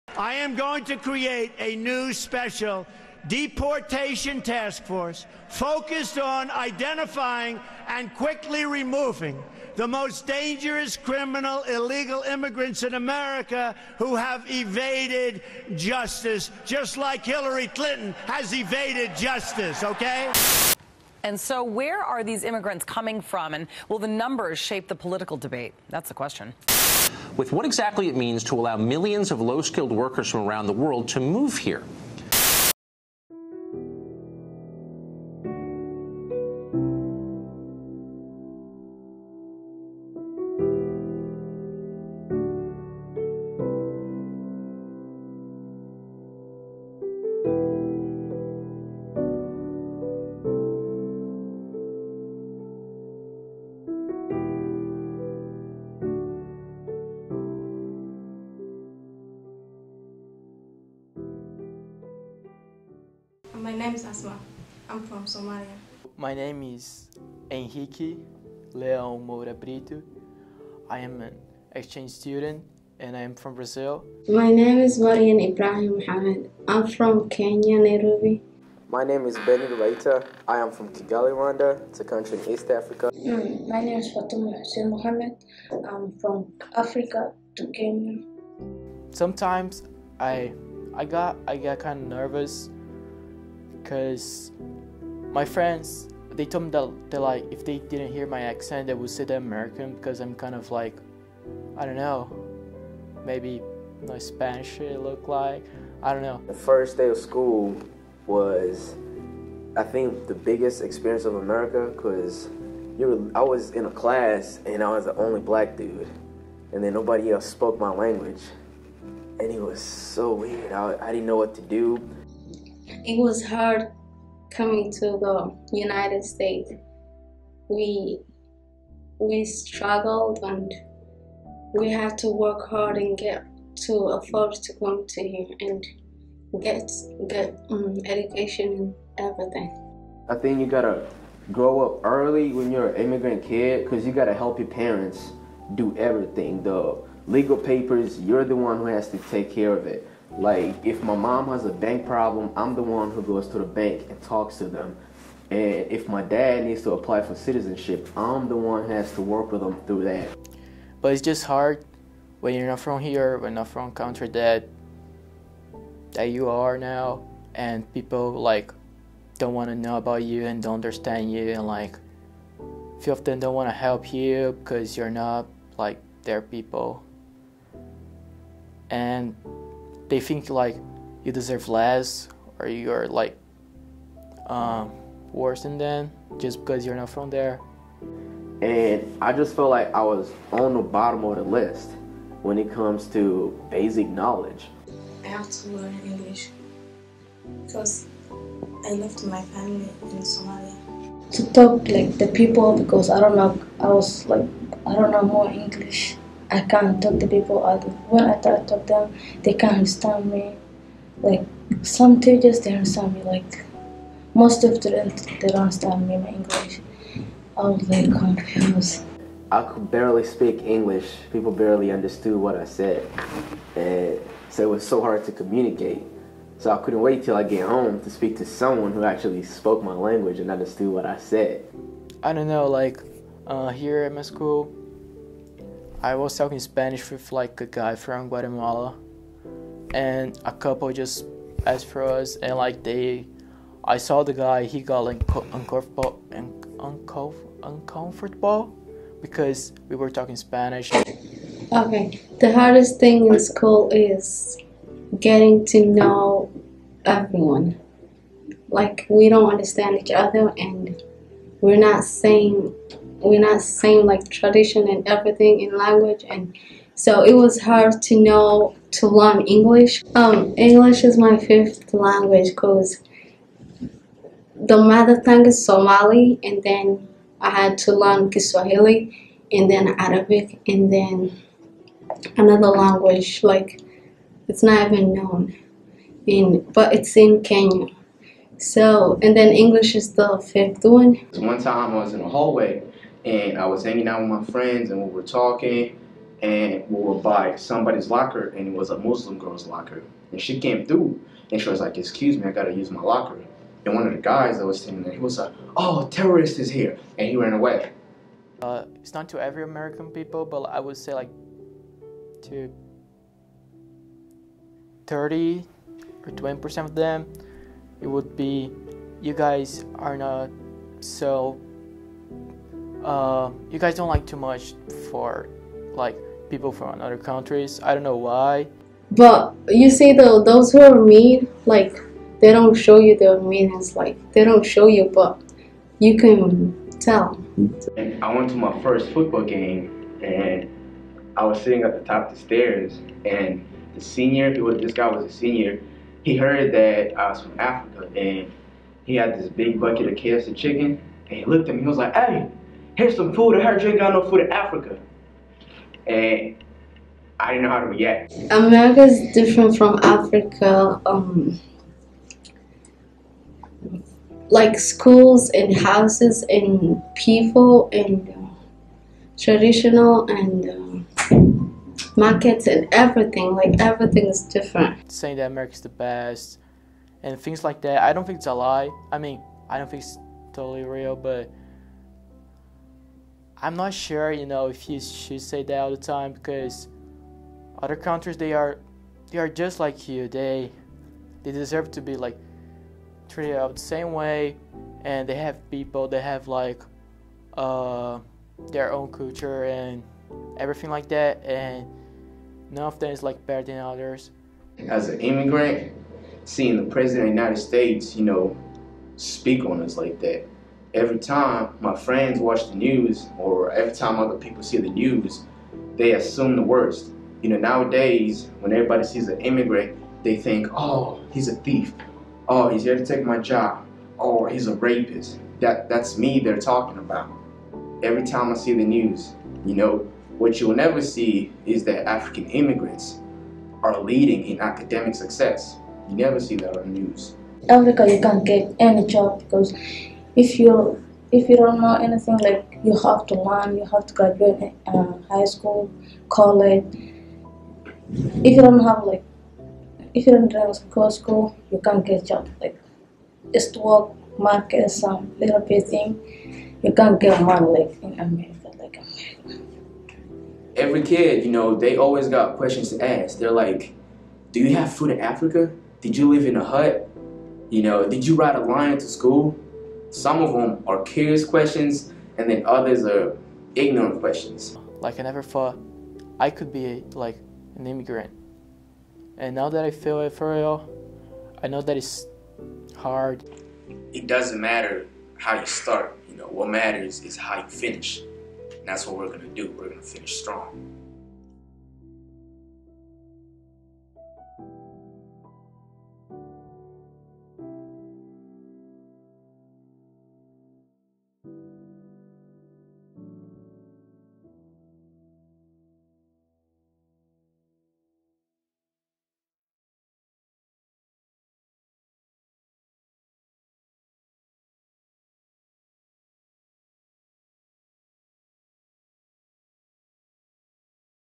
Coming to America (A documentary by Benin250 and AragonMedia)
My buddy and I made this video in high school to help open up people's eyes to immigration and foreign students and their struggles in America.